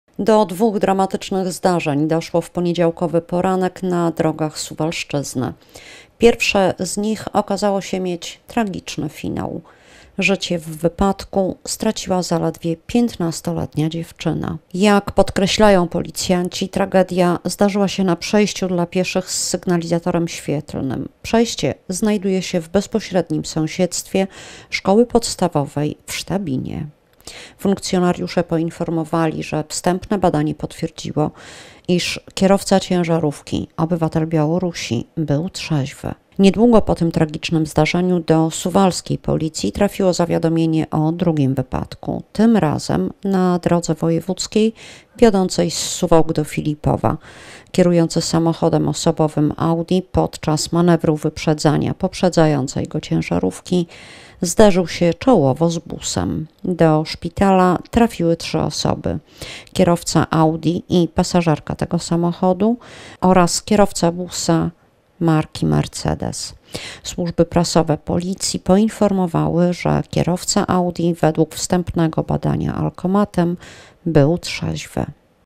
Czarny poniedziałek na drogach |TVSUWAŁKI
Do dwóch dramatycznych zdarzeń doszło w poniedziałkowy poranek na drogach Suwalszczyzny.